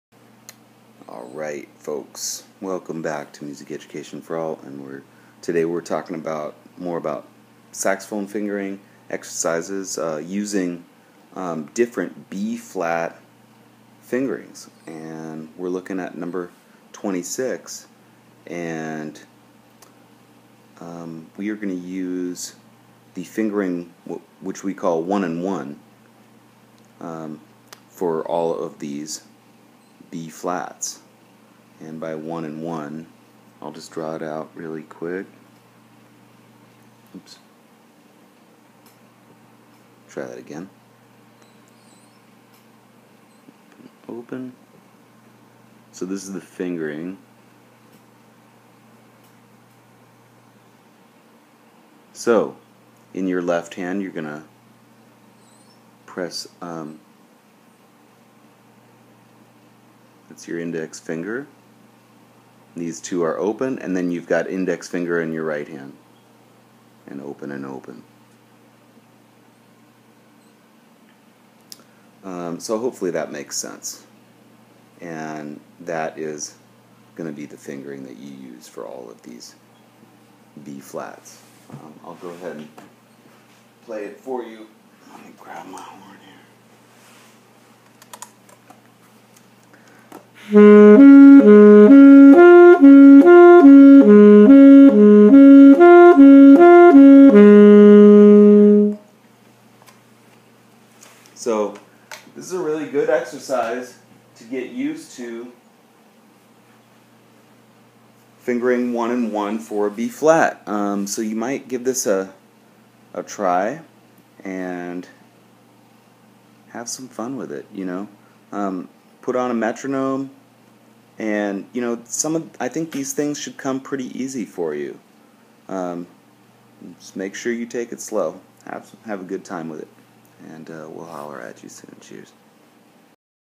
Sax Fingering 1&1 B Flat
Here's a short exercise working on one of the common ways to play B flat on saxophone.